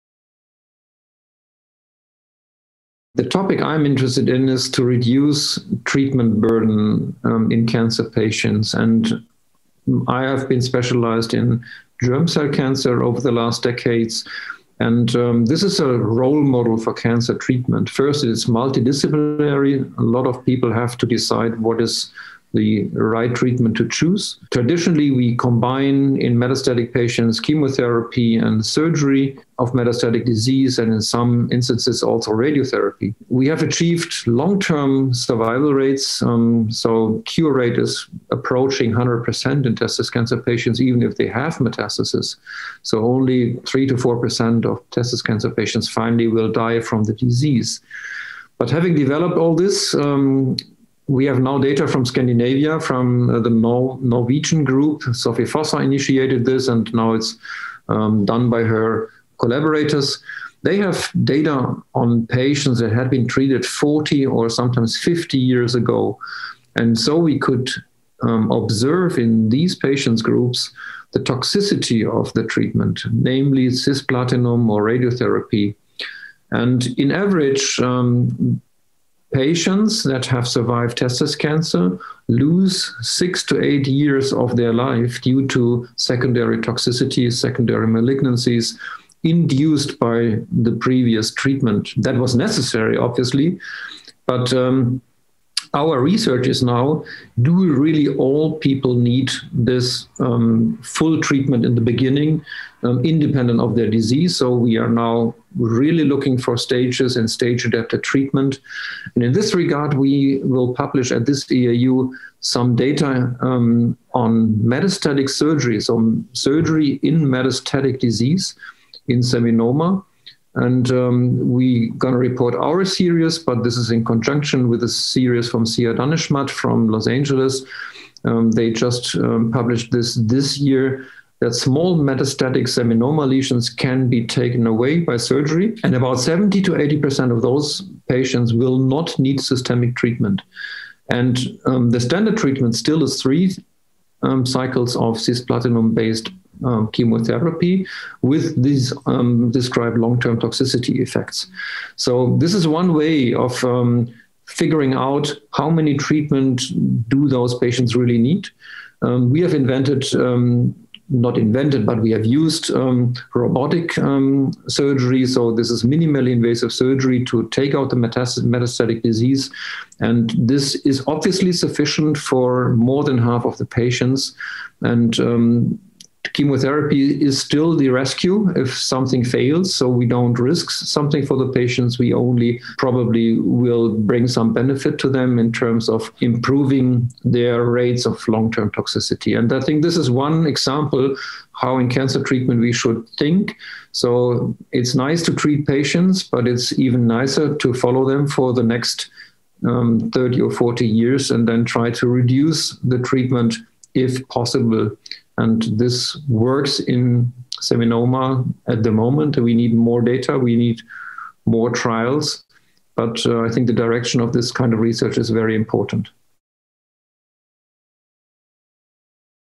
Surgery in metastatic germ cell tumors to reduce disease burden and improve long-term outcomes
Peter Albers, MD, University of Düsseldorf, Düsseldorf, Germany, gives an update on novel approaches to reducing disease burden in germ cell tumors, in particular highlighting long-term survival rates in patients with testicular cancer and long-term outcomes with regards to treatment-related adverse events. Prof. Albers discusses whether surgery in metastatic seminoma can be used to reduce disease burden, thus omitting the need for systemic treatment. This interview took place at the virtual European Association of Urology (EAU) Virtual Meeting 2021.